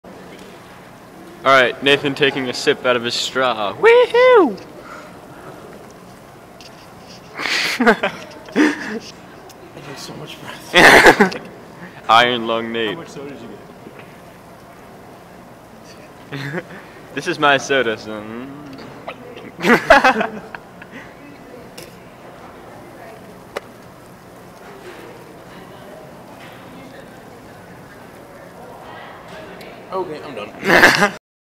Long Straw
Nathan tries to drink a soda out of seven connected straws.